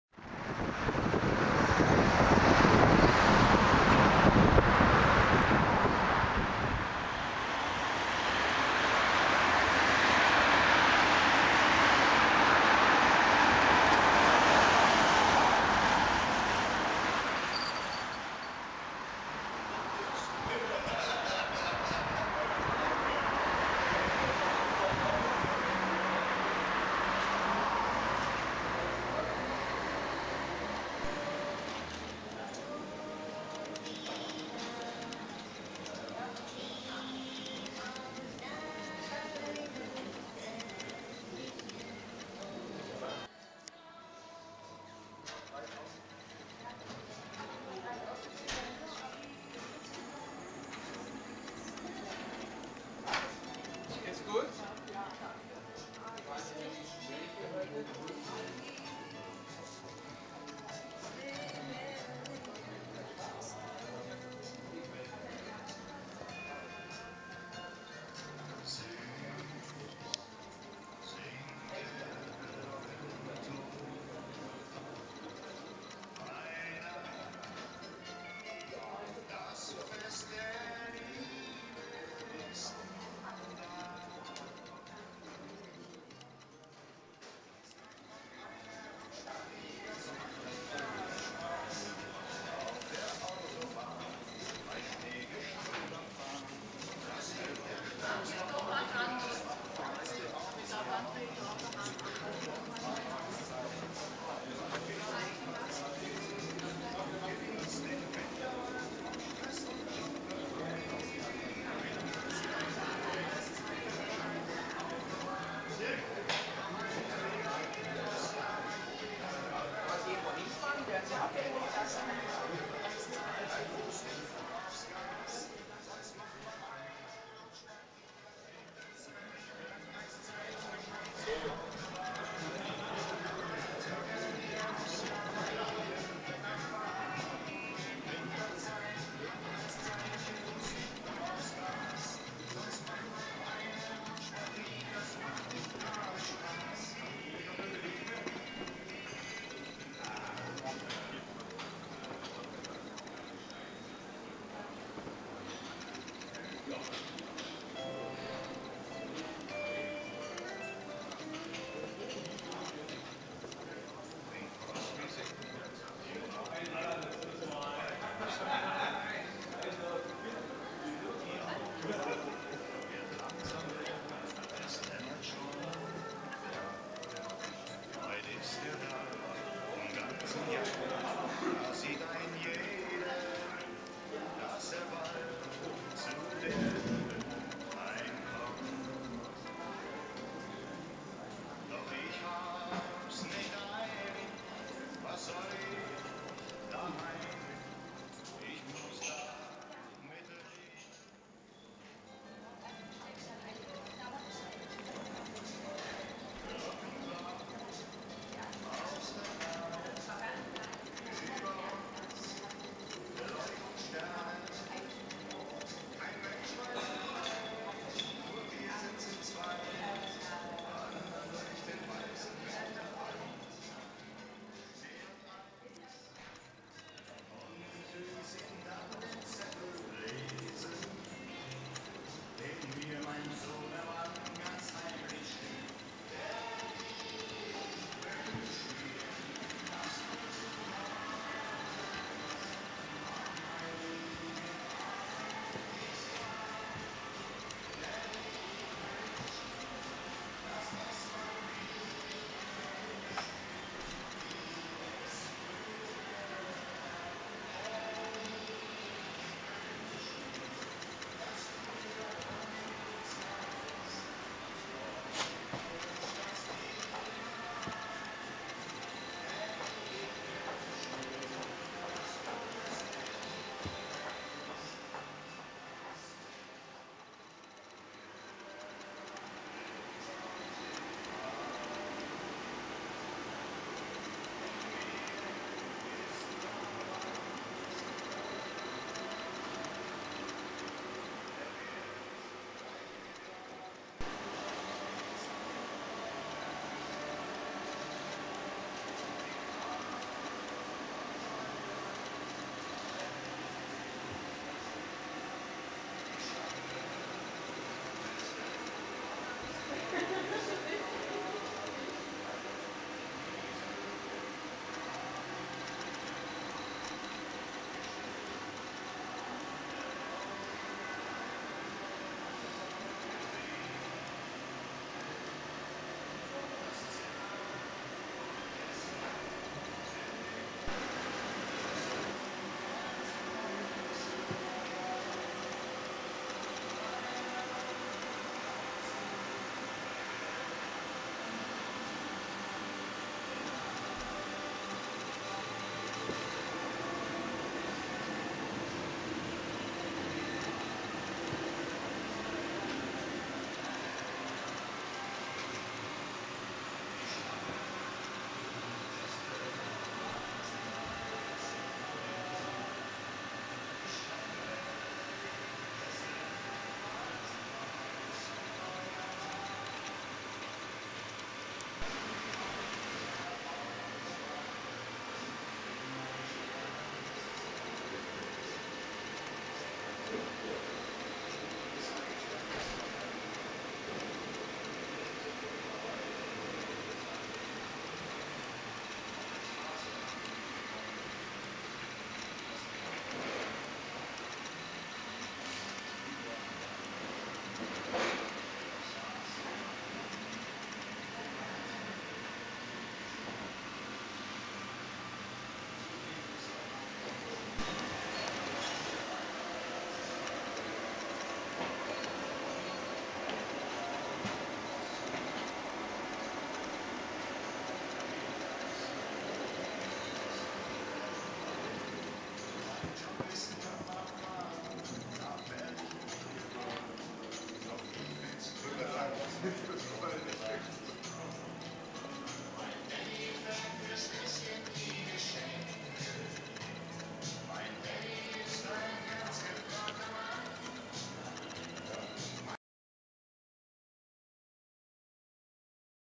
Riesiger begehbarer Christstollen-Karton im MARITIM-Hotel 2016
.. und kleiner Weihnachtsmarkt mit Schwibbögen und Schnitzereien aus dem Erzgebirge im Foyer des MARITIM-Hotel © by Advent TV - unterwegs